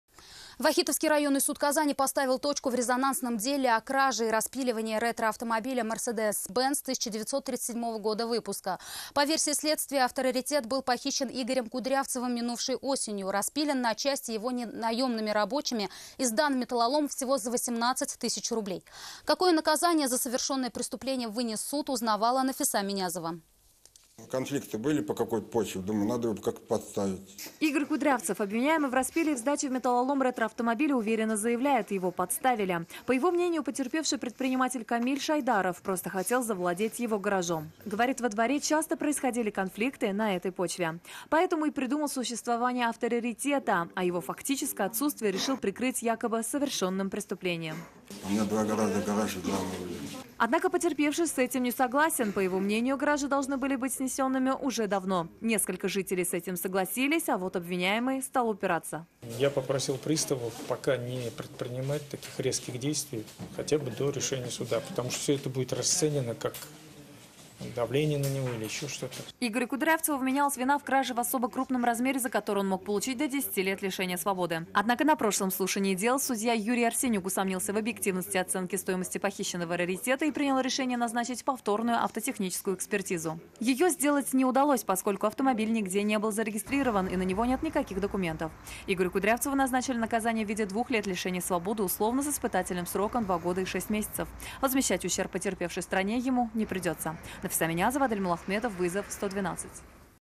Суд Казани поставил точку в резонансном деле о краже и распиле ретроавтомобиля - ТНВ
Вахитовский районный суд Казани поставил точку в резонансном деле о краже и распиле ретроавтомобиля Mersedes-Benz 1937 года выпуска.  По версии следствия автораритет был похищен Игорем Кудрявцевым минувшей осенью, распилен на части его наемными рабочими и сдан в металлолом всего за 18 тысяч рублей. Какое наказание за совершенное преступление вынес суд?

Таяну ноктасы. В эфире: пн, вт, ср, чт - 19:00

7 дней. В эфире: вс - 19:30, 21:30

Координаты:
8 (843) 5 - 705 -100